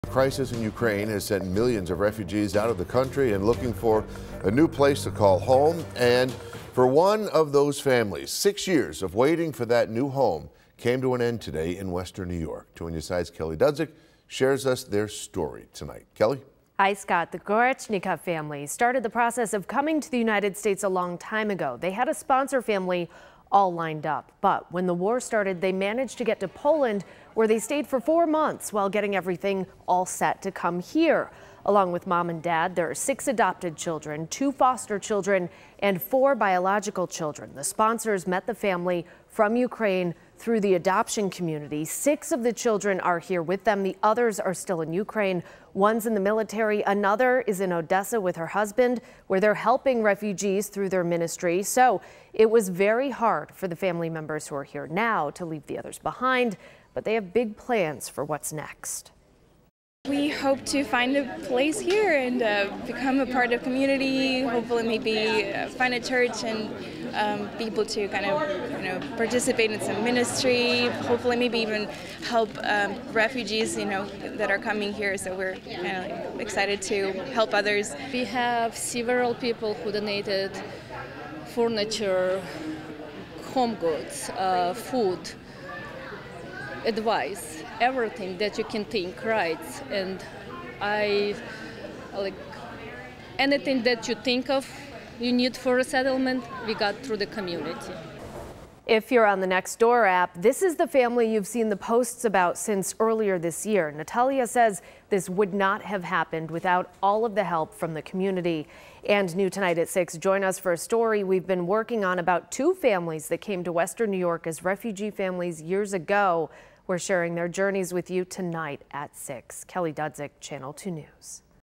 Family Arrives In WNY From Ukraine
The sponsors met the family from Ukraine through the adoption community. Six of the children are here with them... the others are still in Ukraine.